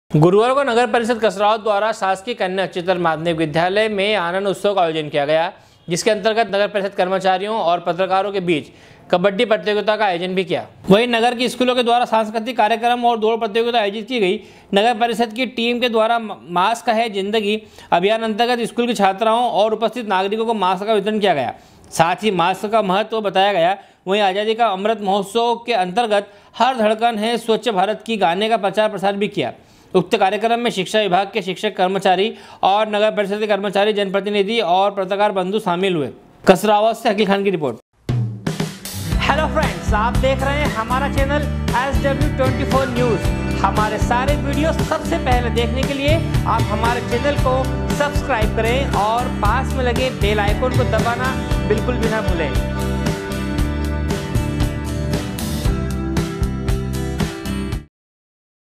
SW 24 News Madhyapradesh
गुरुवार को नगर परिषद् कसरावद द्वारा शासकीय कन्या उच्चतर विद्यालय परिसर में आनंद उत्सव का आयोजन किया गया जिसके अंतर्गत नगर परिषद् कर्मचारियों एवं पत्रकारो के बीच कबड्डी प्रतियोगिता का आयोजन किया गया। कसरावद से अकील खान की रिपोर्ट